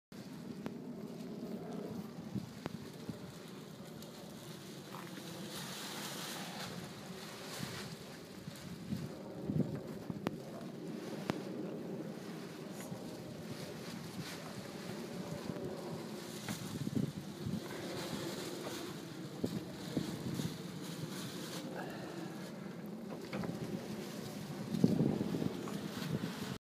Argon's asym flies for the first time
Flying our Doyle Asym for the first time.  We had light air and some time to kill, so we gave ourselves some sea room and practiced for a while. Looks pretty good